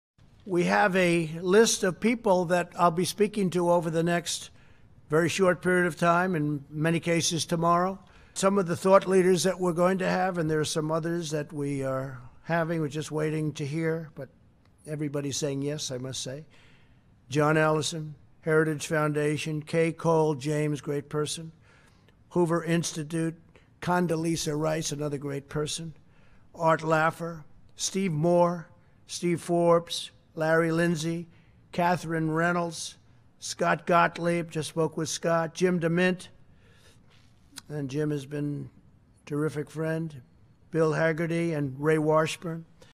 Kay C. James Will Be Part of the Great American Economic Revival
President Trump announced Kay C. James will be a part of the Great American Economic Revival—working to reopenAmerica and begin economic recovery.

We look forward to sharing recommendations from the National Coronavirus Recovery Commission.